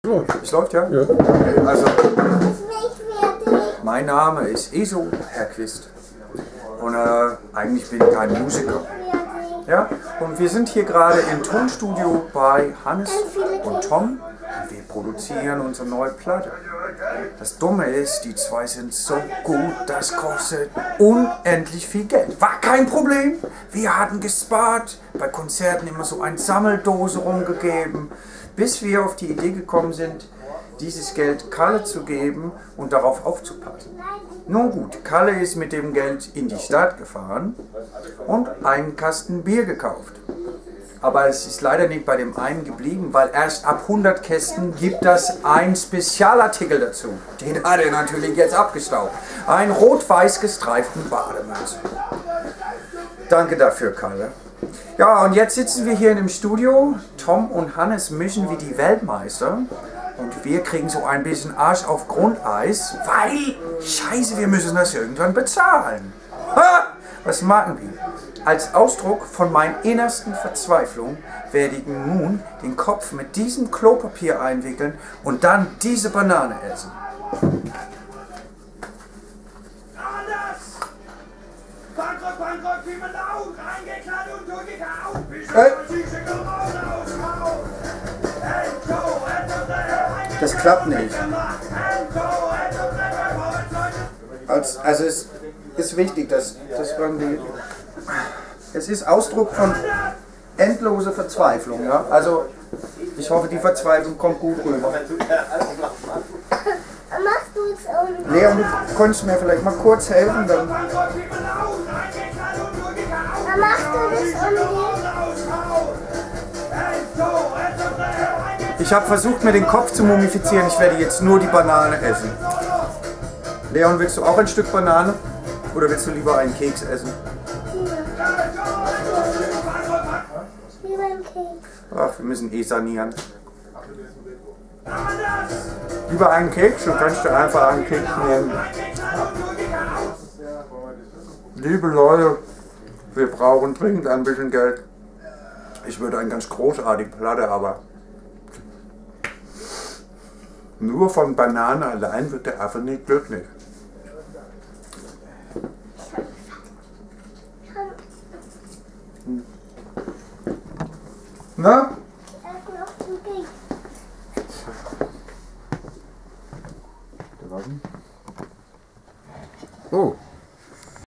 Gebt uns euer Geld (Version 1)
Iso braucht dich. Also dein Geld. Denn sonst kann er die neue Scheibe nicht bezahlen.
Hier die Version 1: Iso wickelt sich den Kopf mit Klopapier ein, isst am Ende aber nur die Banane. Leon möchte lieber einen Keks.